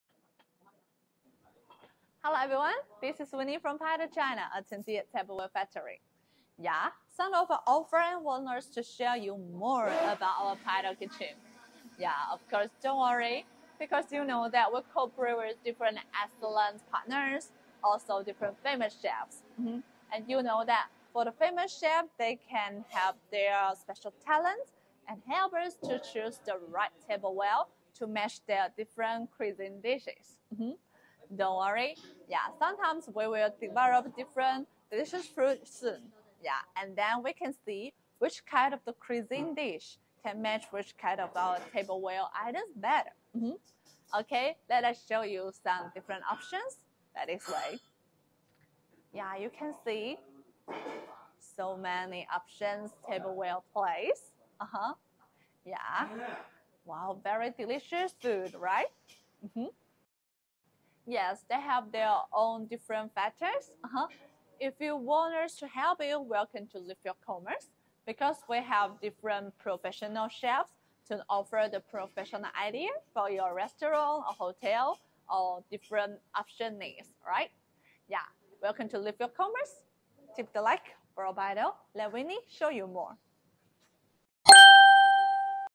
Share PITO’s delicious food
PITO Tableware Company not only sells ceramic tableware, but also has a dedicated team of chefs to match new tableware styles with different delicacies. They adjust the design ideas of tableware through the practice of chefs and consider it from the perspective of consumers.